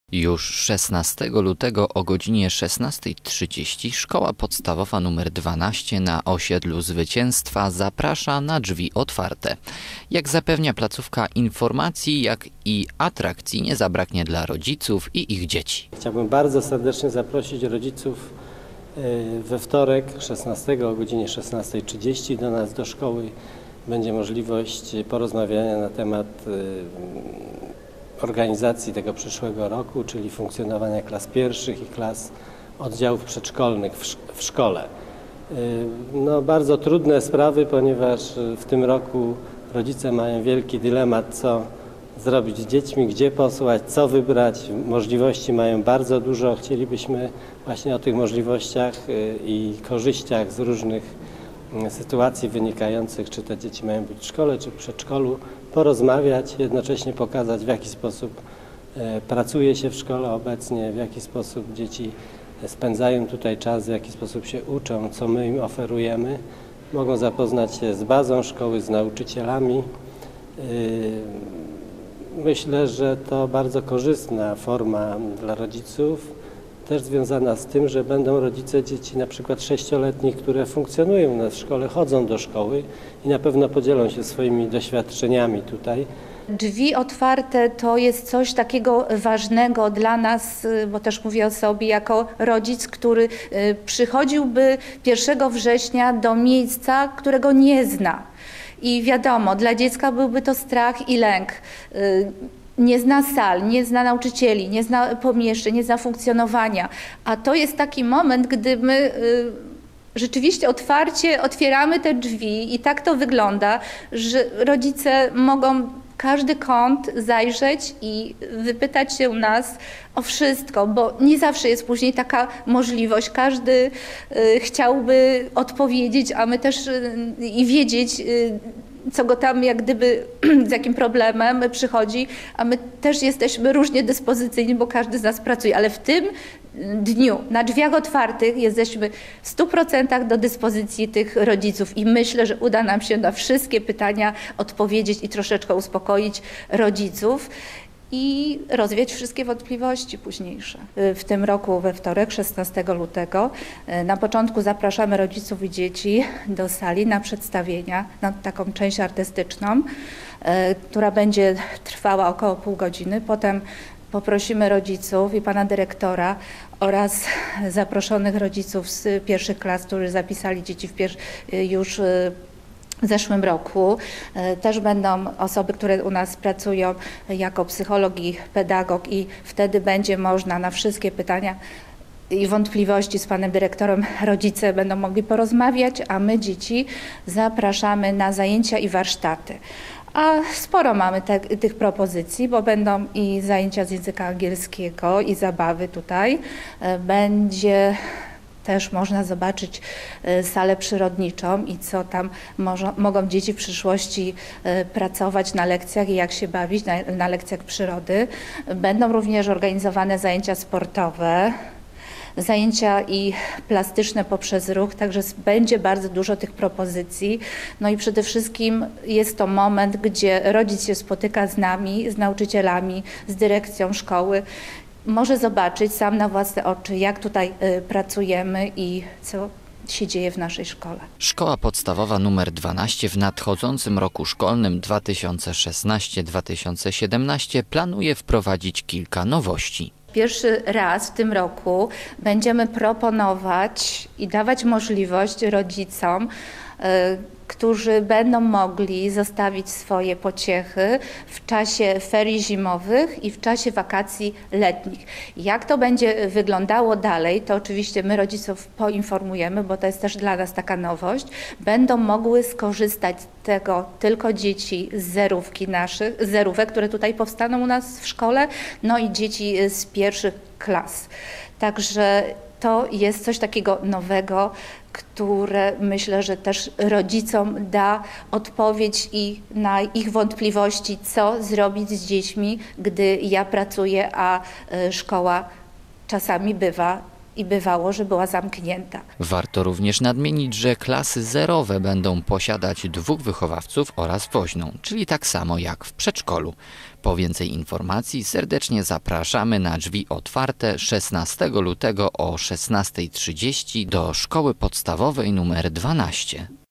2016.02.12 Zaproszenie na „Drzwi otwarte” do Szkoły Podstawowej nr 12
Szkoła Podstawowa nr 12 im. Wielkopolskiej Brygady Kawalerii w Poznaniu

zaprasza dzieci i rodziców
przyszłorocznych uczniów szkoły podstawowej (roczniki 2009 i 2010)
i oddziałów przedszkolnych (roczniki 2010 i młodsze)
na "Drzwi otwarte"

pierwsze spotkanie
16 lutego 2016 r. (wtorek)
godz. 16.30

Materiał filmowy został zrealizowany przez Telewizję WTvK.